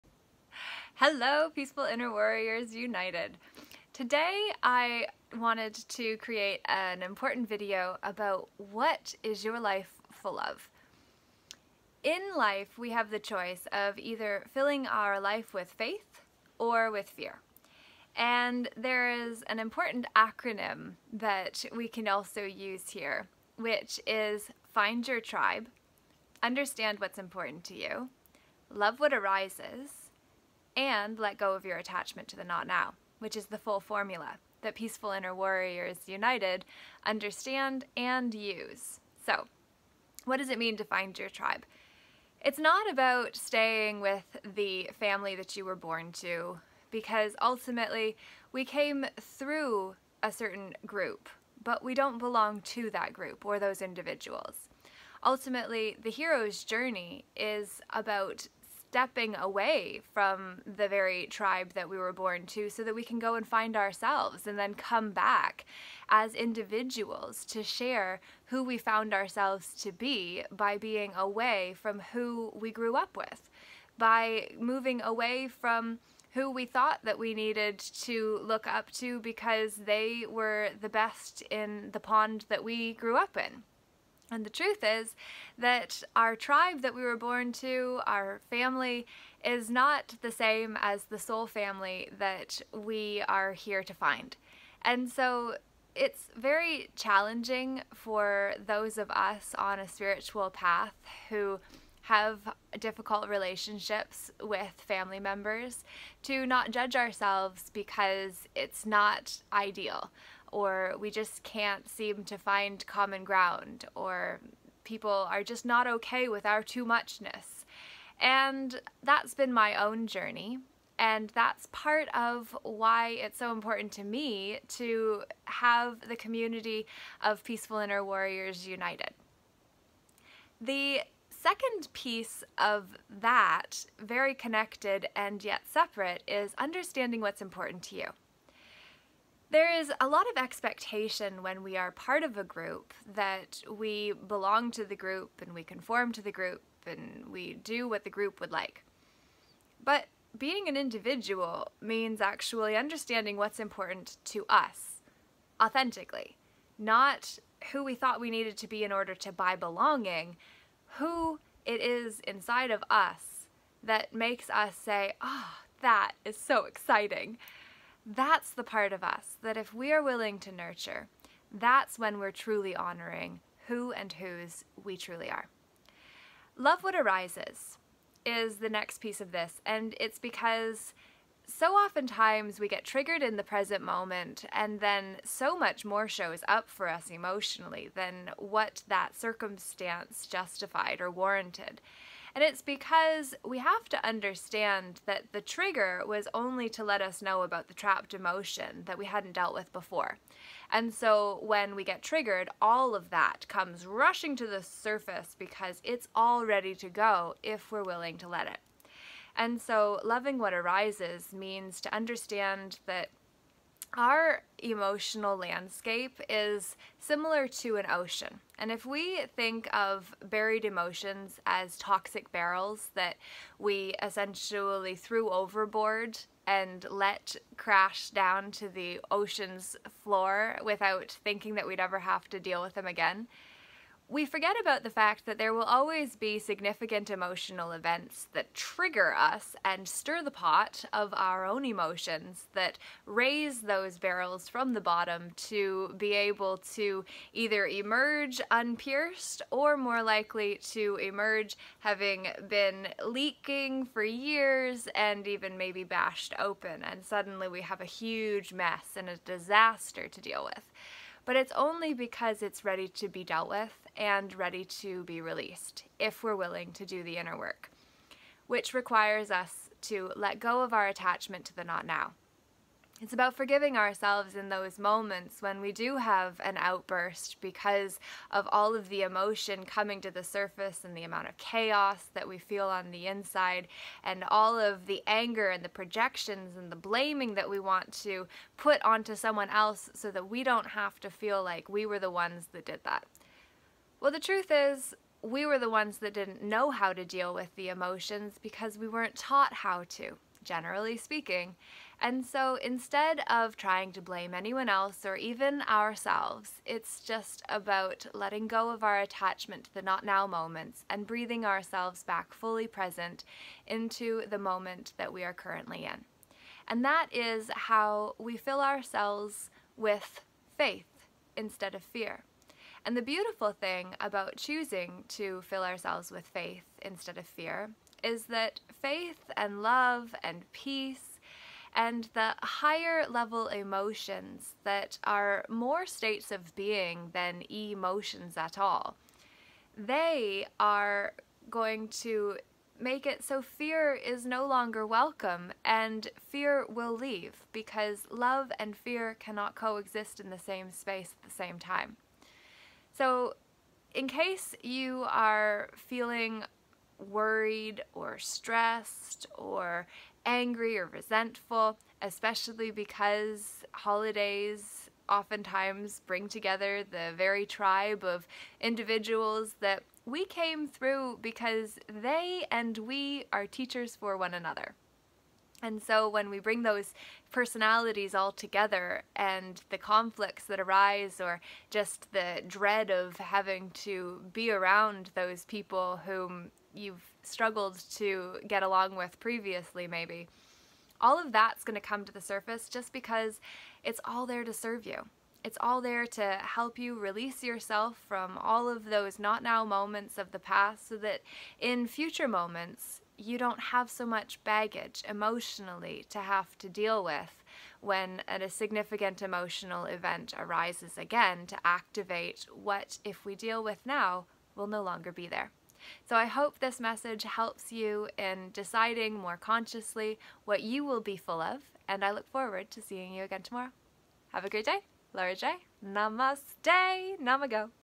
What Are You F.U.L.L.Of & How To Choose Faith Over FEAR
As promised in yesterday’s video, here is the FULL formula that you can use to fill yourself and your life with faith instead of FEAR